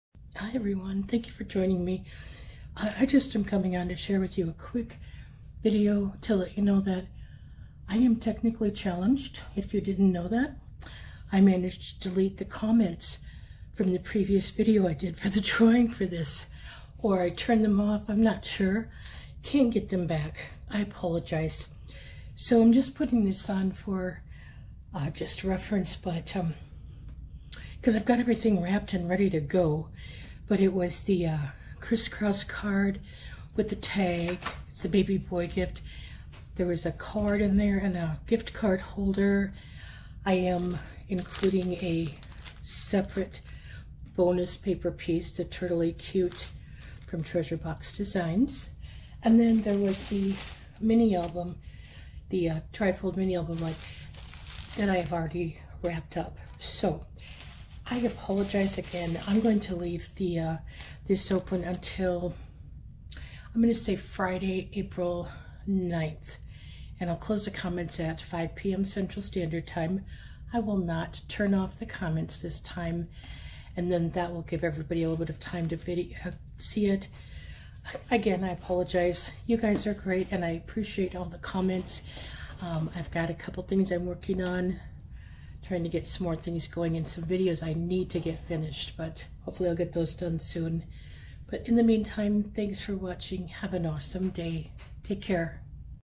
OOPS! Redo of Baby Boy Set Giveaway - Ends 4/9/2021
I managed to lose the comments from the 1st video, so I'm redoing the video.  I'll end the comments for the giveaway on April 9, 2021 at 5:00PM CST.  I'm sorry for the mix up!